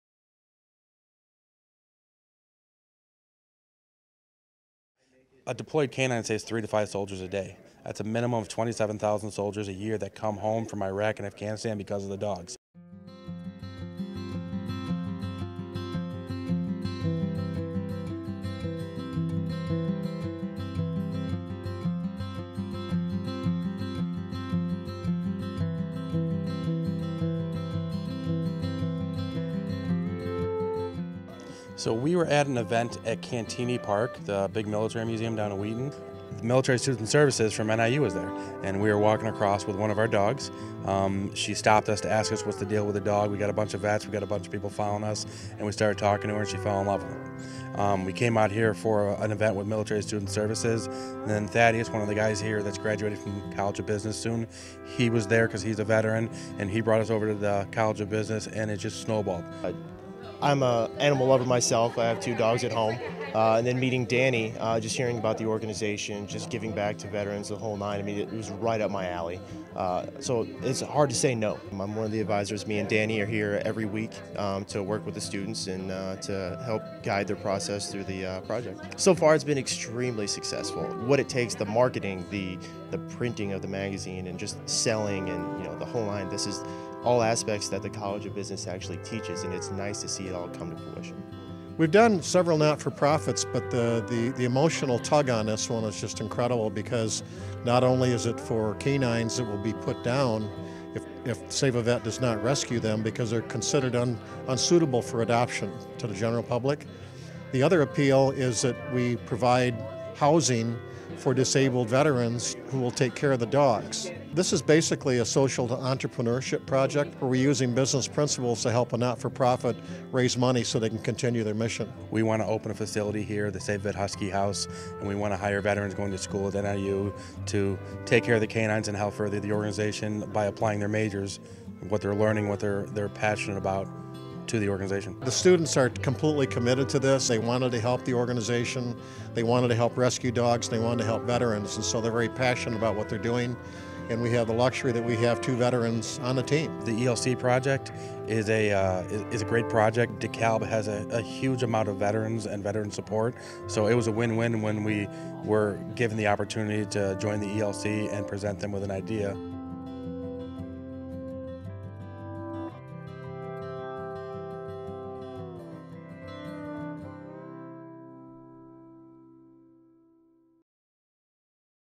NIU students partner with Save-A-Vet
A group of NIU students is working with Save-A-Vet to provide a better life for disabled veterans and military dogs.

Many of the dogs that come to the organization have amazing stories -- they have helped police confiscate millions of dollars in drugs, sniffed out hundreds of roadside bombs in Afghanistan or detected booby-trapped trucks that could have killed hundreds of soldiers. Still, until very recently, the dogs were considered equipment and were often considered unadoptable.

Save-A-Vet, which is based in Lindenhurst, Ill., also serves disabled military, law enforcement and first responder veterans who receive housing in exchange for taking care of retired K-9s.